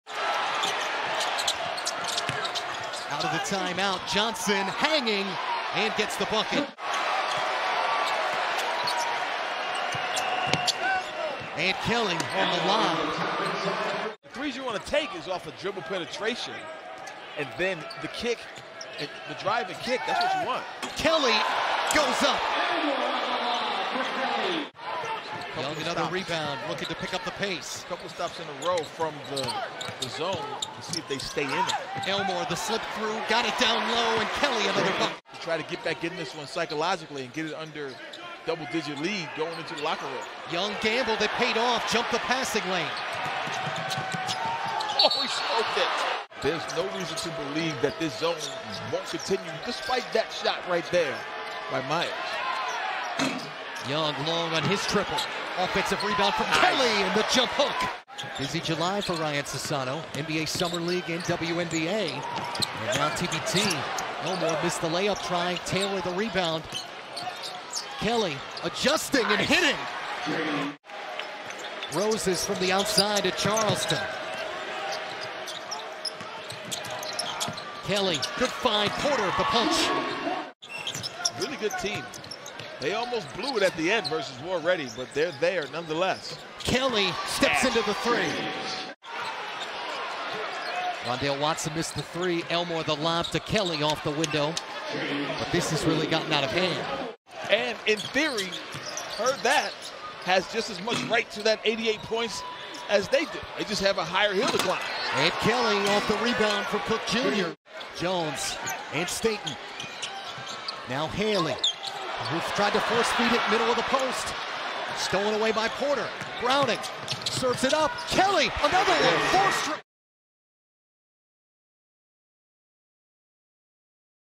James Kelly (23 points) Highlights vs. Best Virginia
James Kelly (23 points) Highlights vs. Best Virginia, 07/26/2022